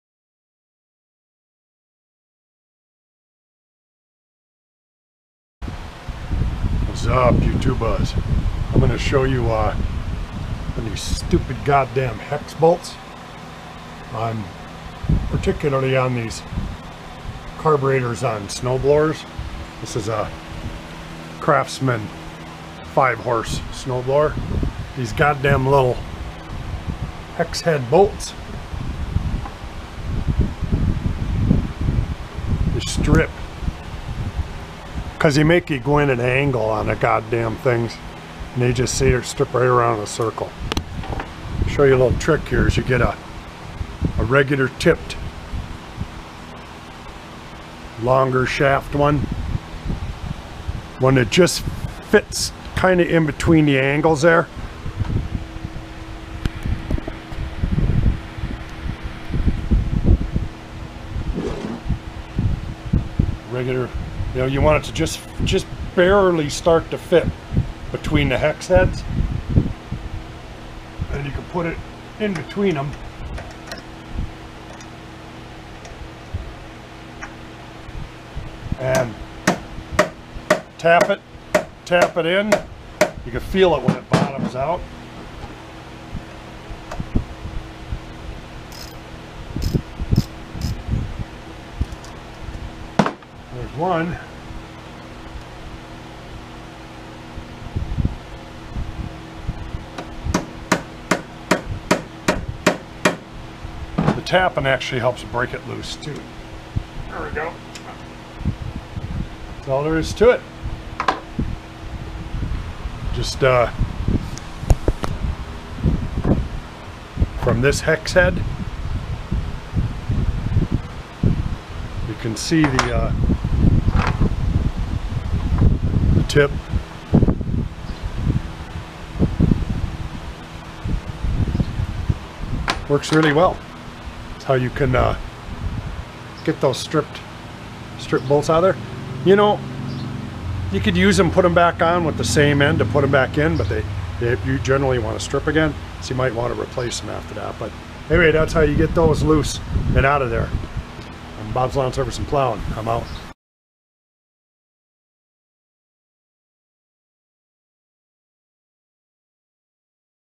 HOW TO REMOVE STRIPPED TORX OR HEX BOLTS ON CARBURETORS
IN THIS VIDEO I WILL SHOW HOW TO REMOVE STRIPPED TORX OR HEX BOLTS. ON CARBURETORS,CAR VARY EAZY.  SUB, COMMENT, LIKE.